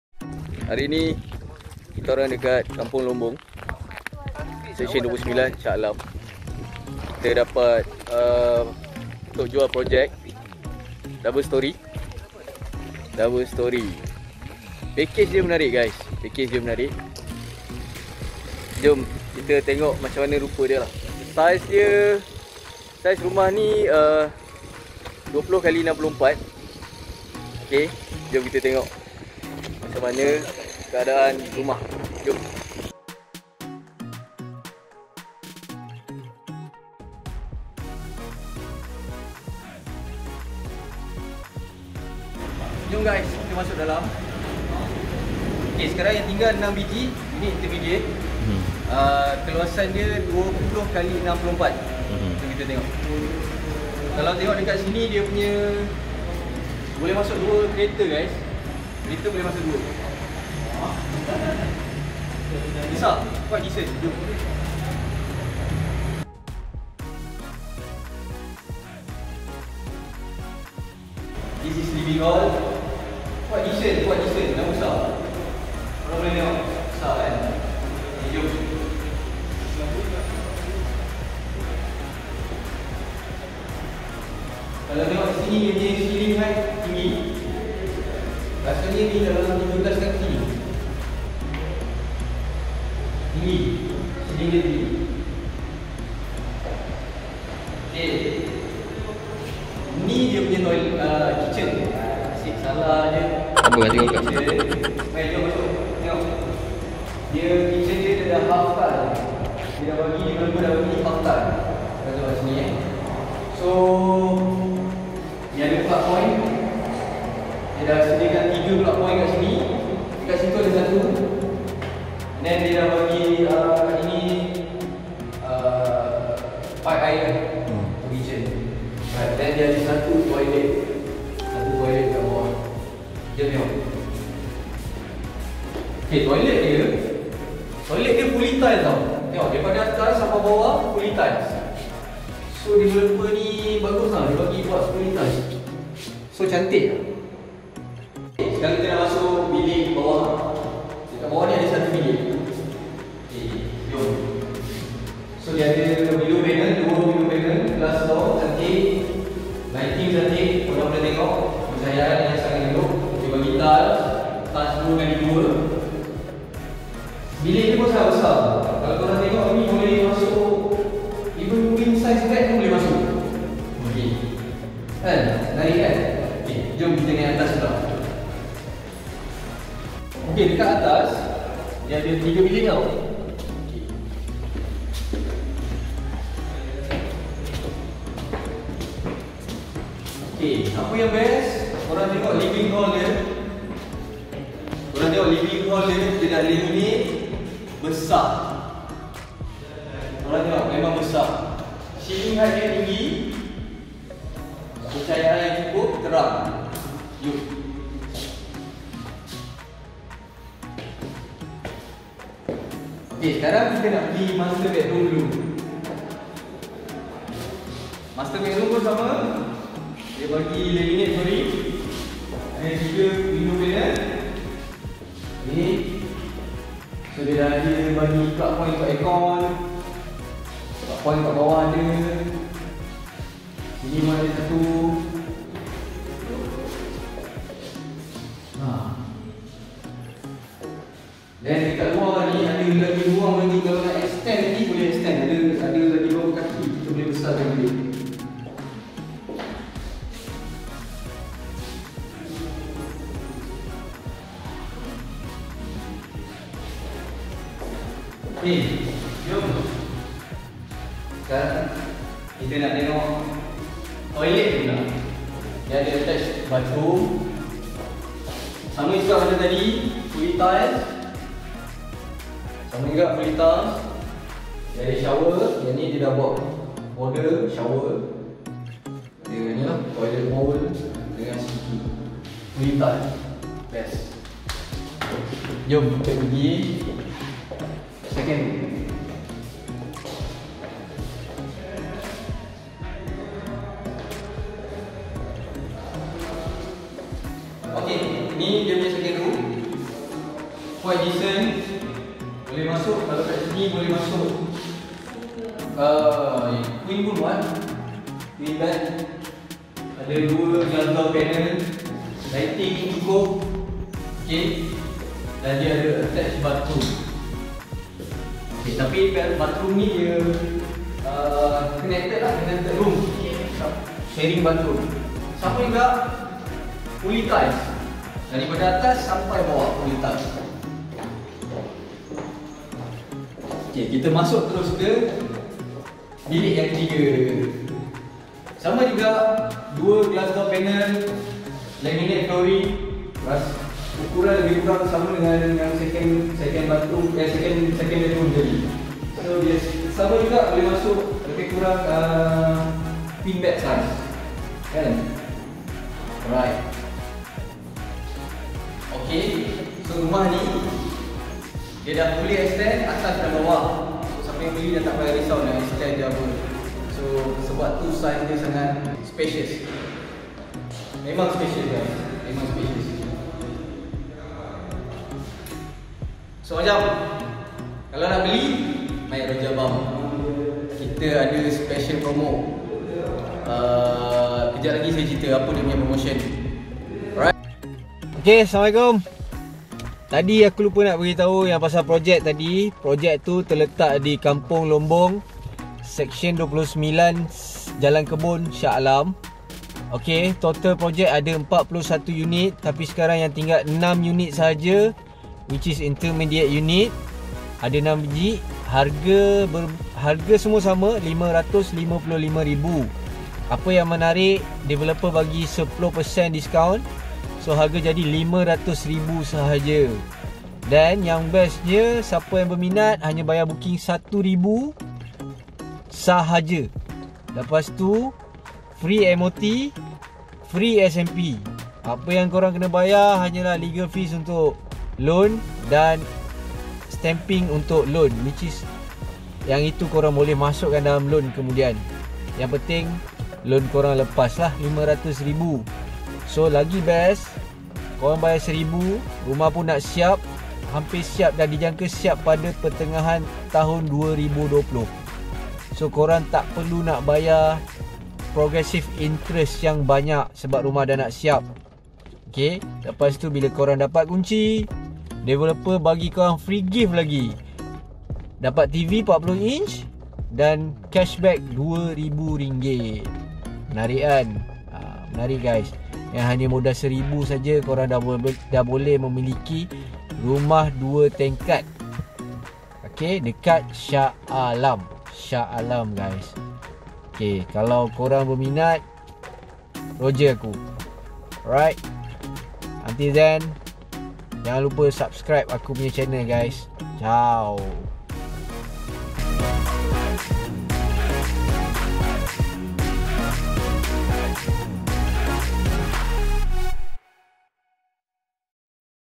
BAYAR RM1000 SAHAJA DAPAT RUMAH 2 TINGKAT DI SHAH ALAM
BOOKING RM1000 SAHAJA‼️TERES DI JALAN LOMBONG JALAN KEBUN‼️ FREE SPA, MOT‼️ RM500k SAHAJA‼️ RM500k‼️

PROJEK BARU RUMAH TERES DUA TINGKAT

Sekysen 29, Shah Alam

Tawaran istimewa:
*Land Area : 20x64sqft
*Built Up : 1875sqft
*Bedrooms : 4
*Bathrooms : 3
*Laminated Timber Flooring at 1st Floor
*Family Area at 1st Floor
*Percuma TV LCD 40inci
*Percuma Legal Fees SPA
*Percuma MOT

SPA Price : RM555,555
Rebate 10% RM55,555

Nett Price After Rebate : RM500,000.

✅Booking Deposit RM1000
✅Zero Downpayment
✅Extra Cash Back upon VP: RM2000

Nett Price After Rebate 10% & Cash Back Rebate RM2K = RM498,000

Mr Shukri
Oriental Realty Shah Alam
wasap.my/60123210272